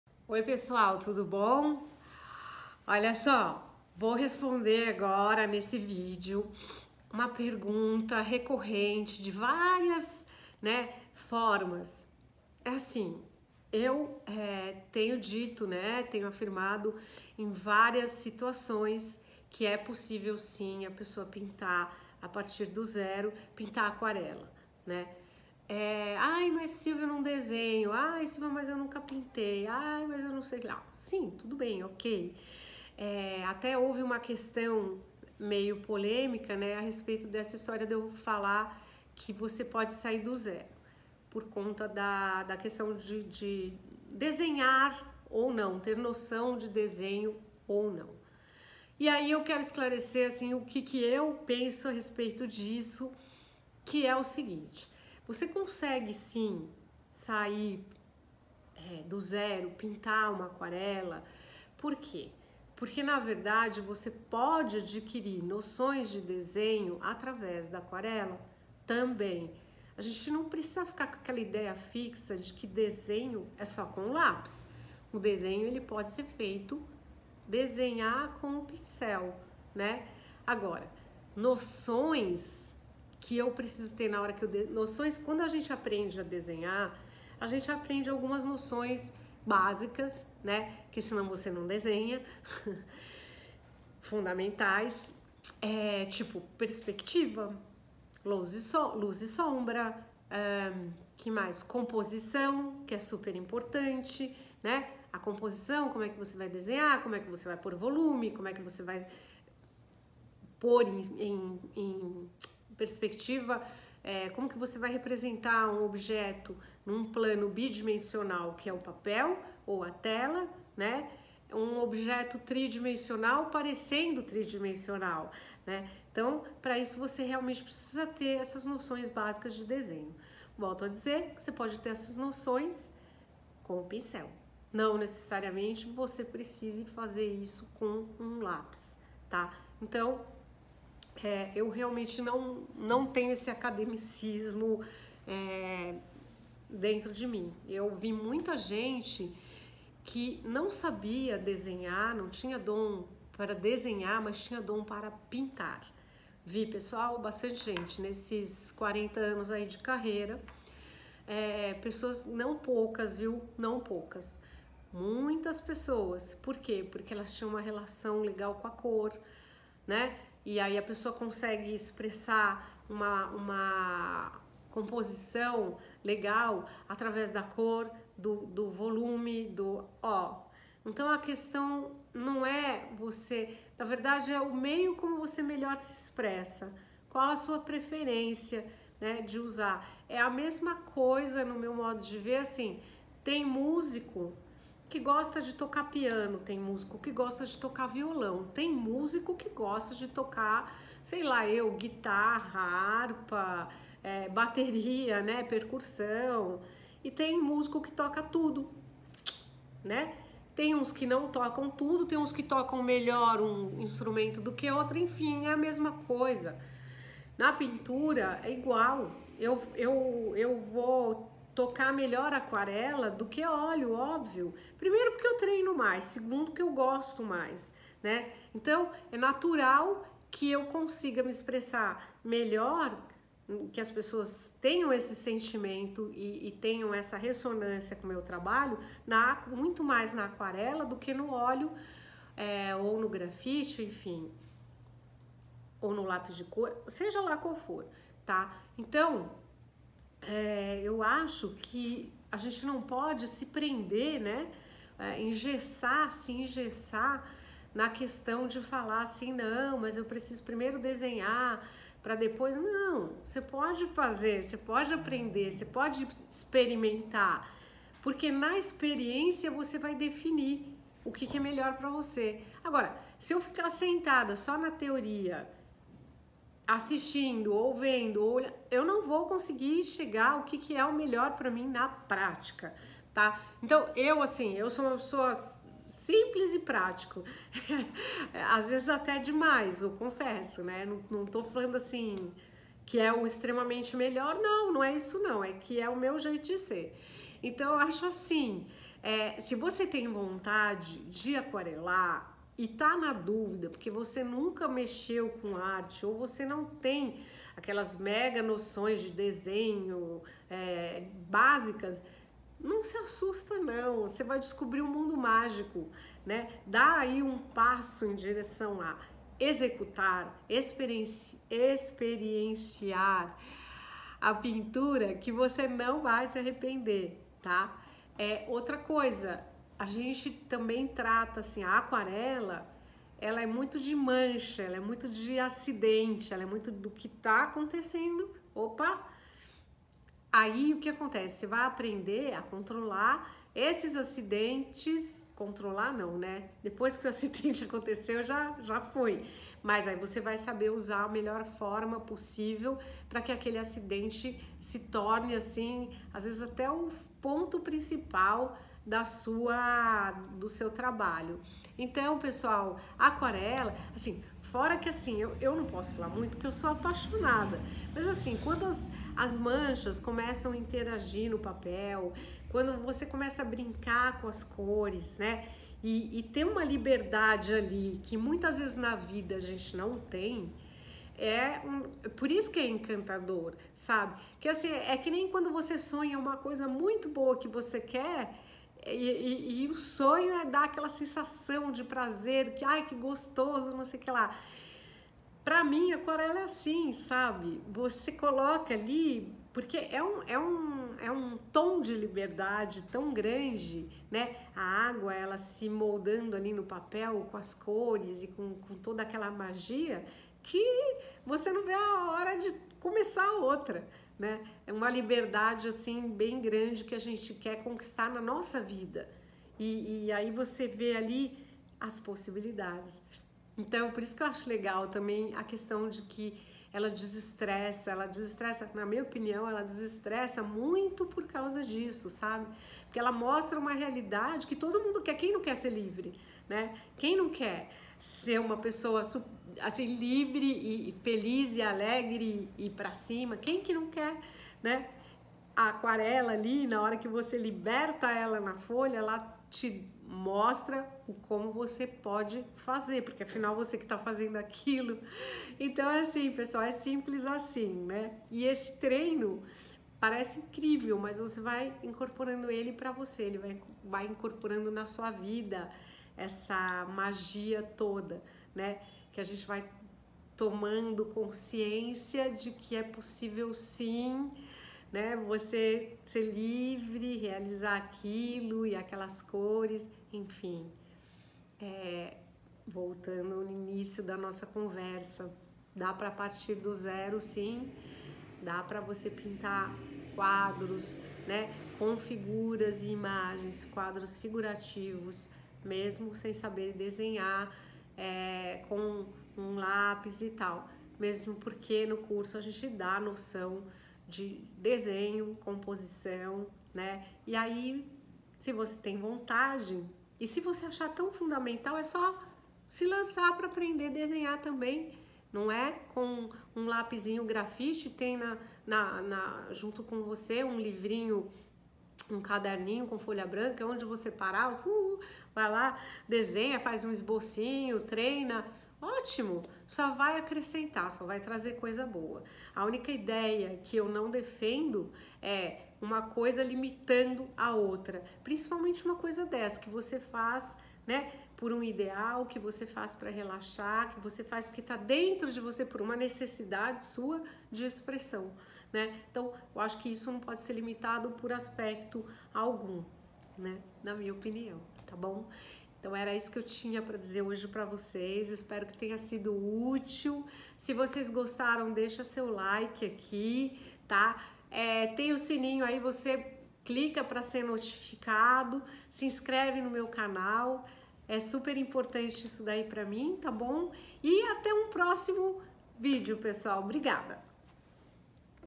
Arte saindo do zero, é possível?
Olá pessoal! Começo essa discrição fazendo uma pergunta: entrar no mundo da Arte sem qualquer dom e saindo do zero é possível? Você que me acompanha deve estar se perguntando, Silvia você já não falou sobre isso? E a resposta é sim! Nestes anos todos vivendo da arte, ficou muito claro que o maior sabotador de grandes potenciais artistas são eles mesmos. Por esse motivo, sinto que devo mostrar de todas as maneiras possíveis que todos podem aprender Arte, pois ela é para todos.

Assistam essa conversa até o final e deixem a sua opinião e dúvidas, vamos trocar idéias e tirar os obstáculos da frente!

⚜ Silvia :)